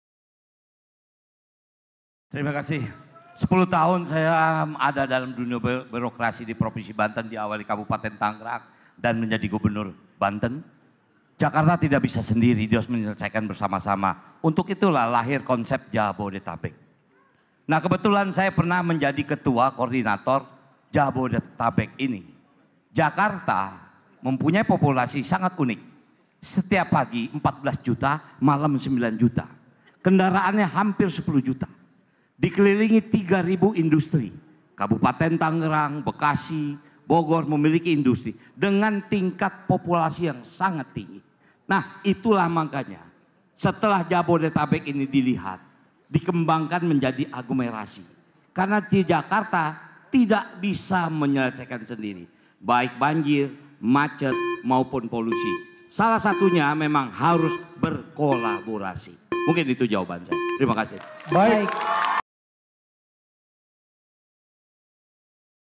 RANO KARNO: JAKARTA TIDAK BISA SENDIRI ATASI POLUSI UDARA
Tanggapi soal fakta di 2023 Jakarta masuk dalam kategori udara terburuk di Dunia, Si Doel ( Rano Karno ) ungkap Jika Jakarta tak pernah bisa atasi polusi, karena Jakarta tak bisa Sendiri, Jakarta dikelilingi Industri, untuk itu perlu Kolaborasi untuk atasi masalah polusi di Jakarta.